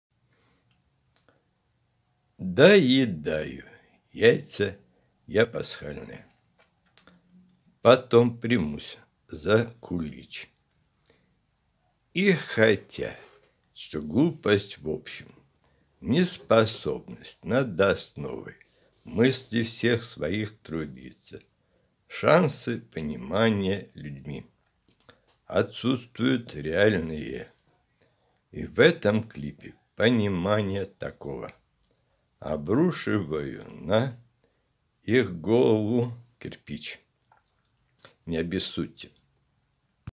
Доедаю яйца я пасхальные
Потом примусь за кулич.
И хотя, что глупость, в общем, — неспособность над основой мыслей всех своих трудиться, шансы понимания людьми отсутствуют реальные,
И в этом клипе понимания такого обрушиваю на их головы кирпич.